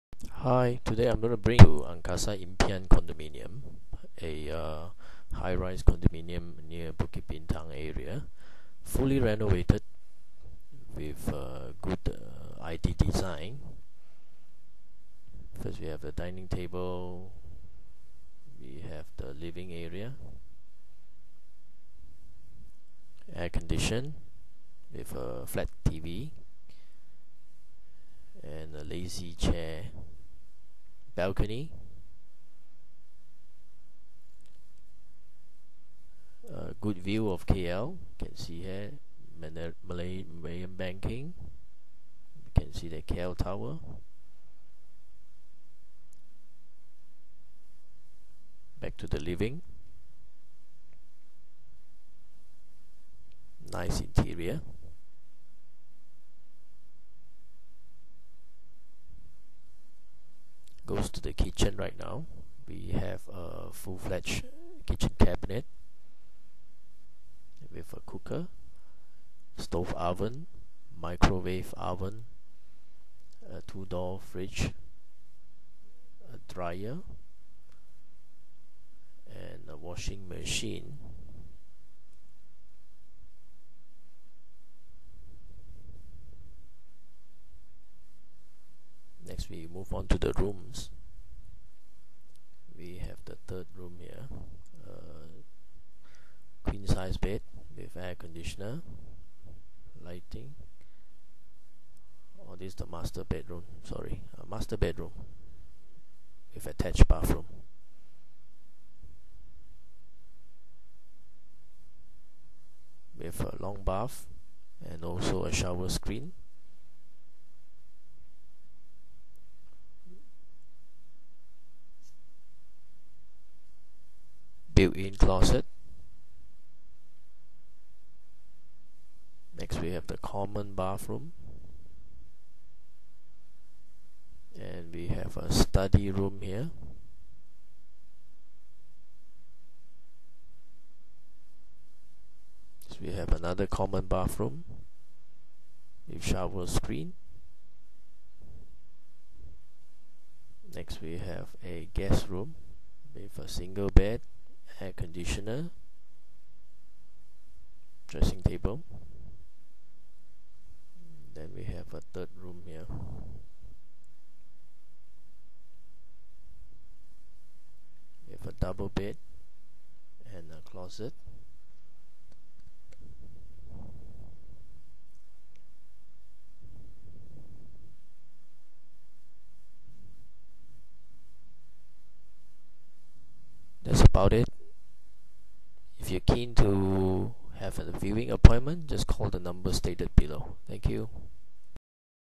ANGKASA IMPIAN CONDO VERY NICE!! FOR RENT CALL 012 3661922
ANGKASA IMPIAN CONDOMINIUM BUKIT BINTANG , HI FLOOR, UNIT , NO VIEW OBSTRUCTION, READY TO MOVE IN , JUST RENOVATED WITH WASHER , DRYER, KITCHEN CABINET, AIRCONS , WATER HEATER, FLAT TV, EASY ACCESS TO BUKIT BINTANG, MONORAIL, SHOPPING AND FOOD HAVEN